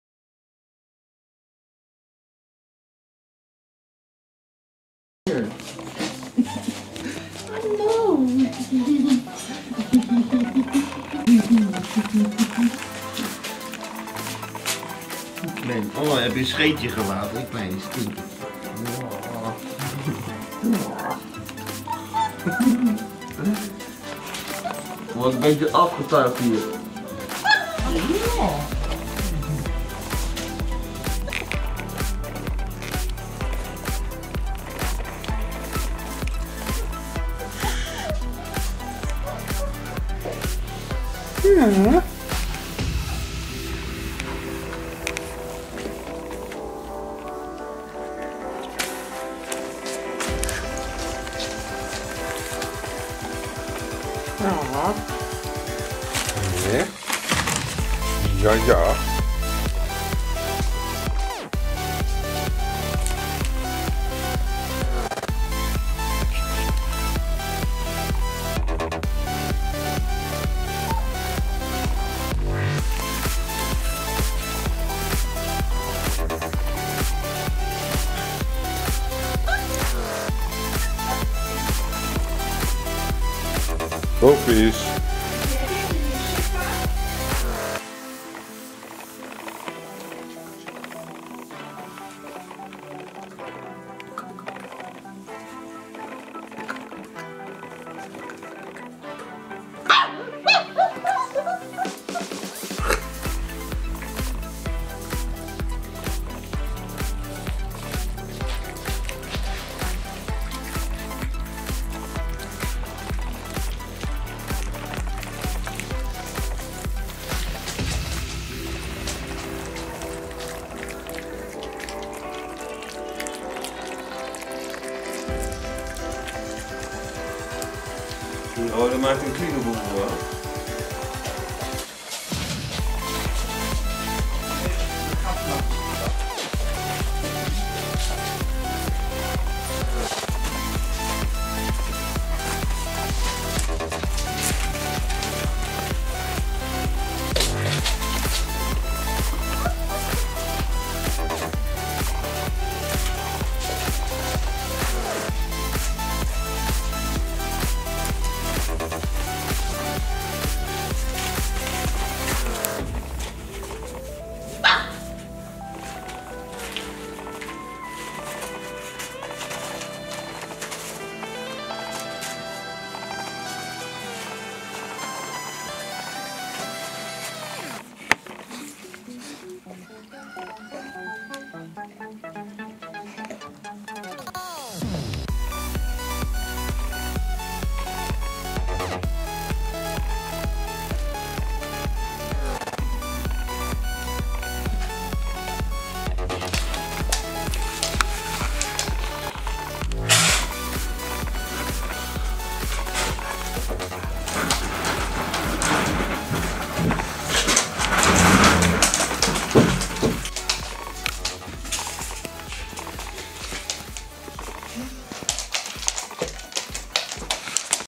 Chiva x Hopper Puppies 28 dagen
Chiva x Hopper Puppies 28 dagen oud, spelen, ravotten, de wereld voor hun word steeds groter,kijk en geniet van het mooie viertal.